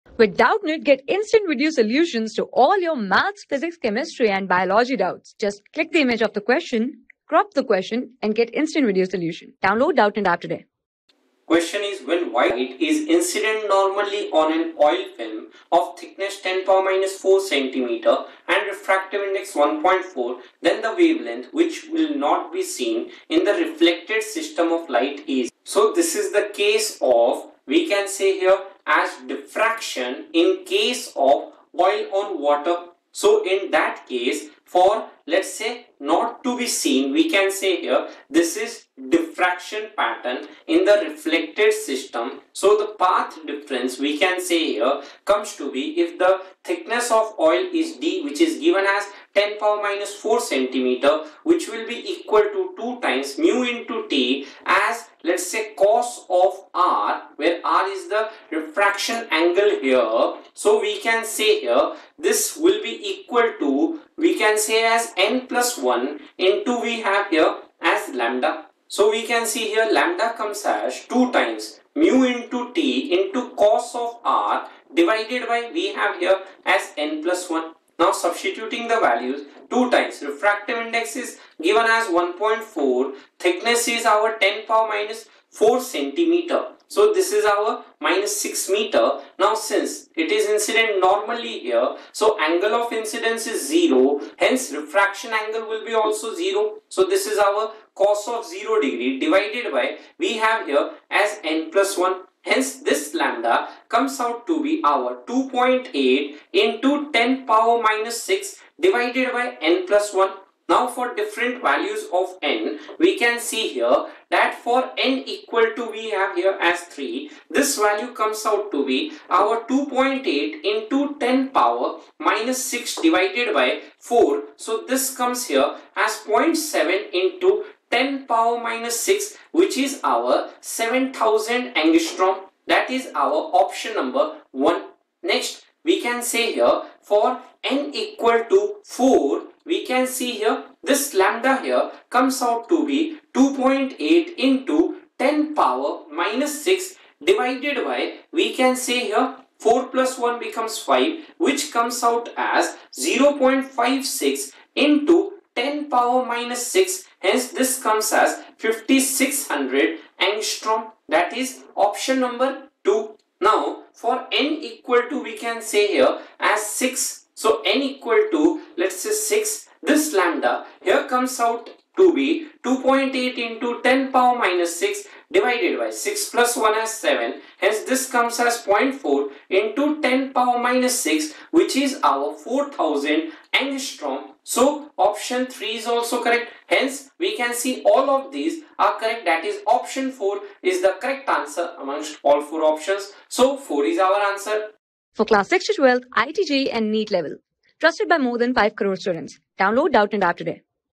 When white light is incident normally an oil film of thickness 10^-4 cm and refractive index. 1....
When white light is incident normally an oil film of thickness 10^-4 cm and refractive index. 1.4 then the wavelength which will not be seen in the reflected system of light is
Class: 12
Subject: PHYSICS
Chapter: WAVE OPTICS
Board:IIT JEE

👉 Have Any Query? Ask Us.